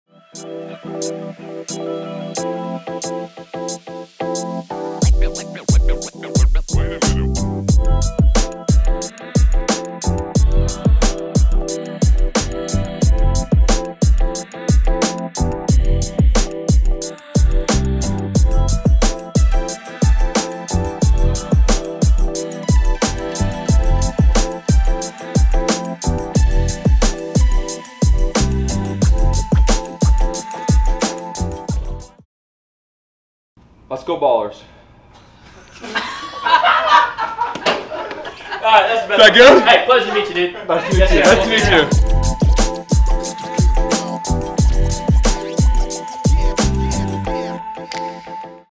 Cannon Ballers 2023 Media Day BTS
Take a peek at what Media day was like for the Future stars of the Chicago White Sox